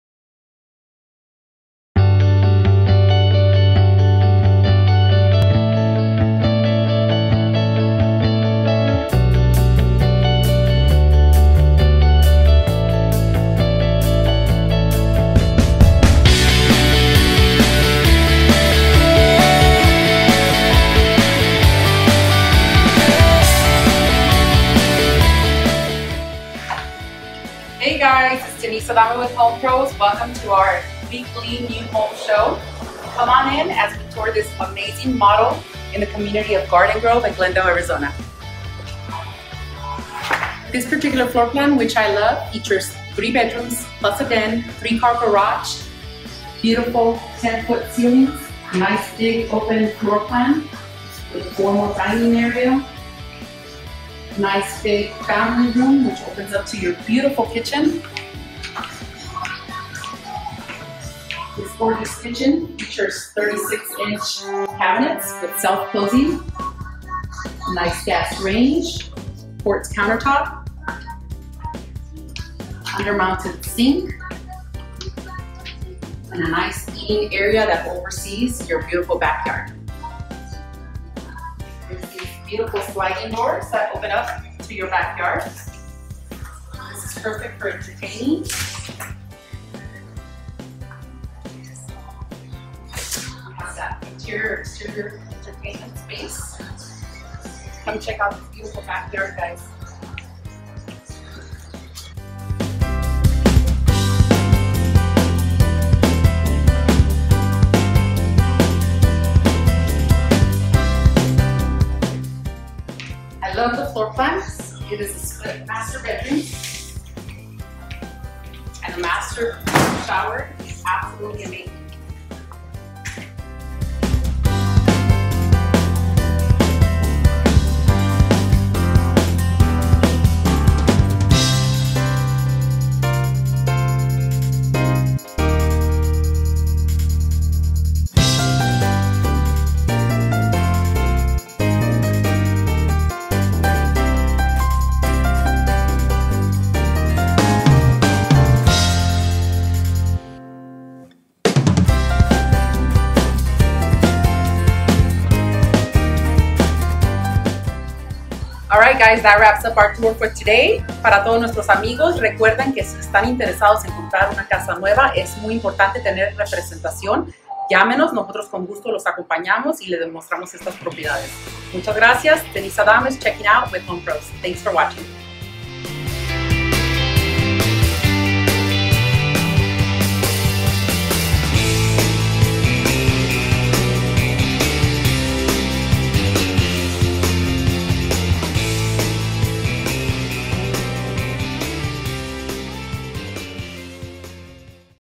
New Home Tour Week 2 / Glendale Arizona
This week Denise is taking us to see this new community called Garden Groves... the location of this community is perfect, just  2 miles away was from the Westgate entertainment district (Bars, restaurants, Movie Theater, Outlets, Hotels,) and access to the 101 Freeway and about 3 miles away from the new resort and casino Desert Diamond that it is still under construction ...